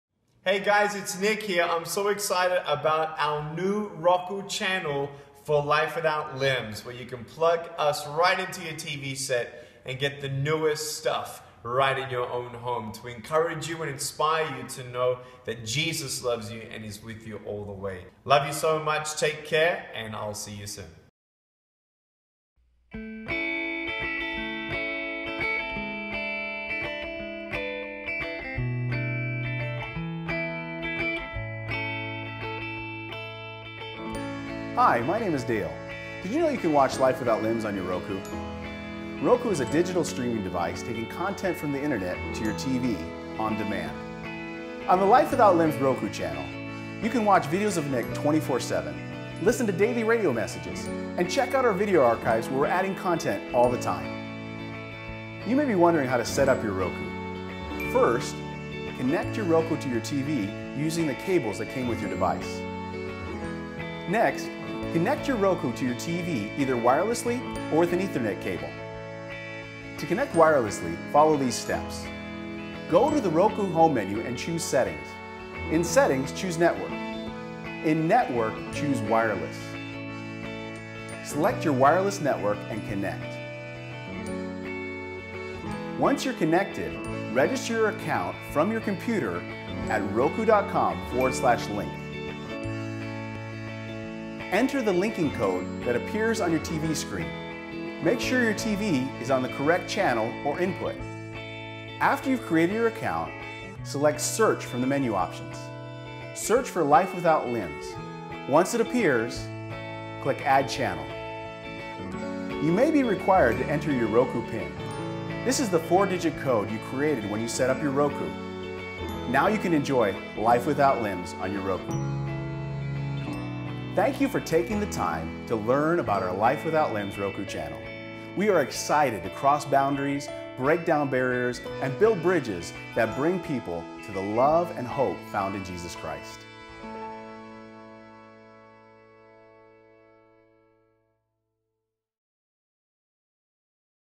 Nick Vujicic on Roku | NickV Ministries
NickV Ministries is now on Roku! Watch the trailer to find out how you can have access 24/7 to our streaming videos, daily radio podcasts and more!